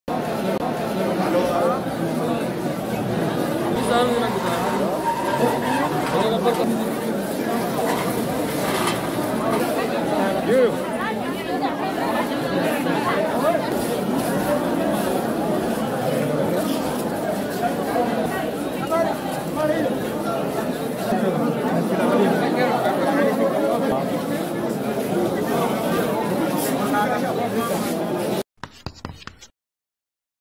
Sudan hospitals overcapacity as fighting continues
Scenes inside the MSF-supported South Hospital in  El Fasher, North Darfur, Sudan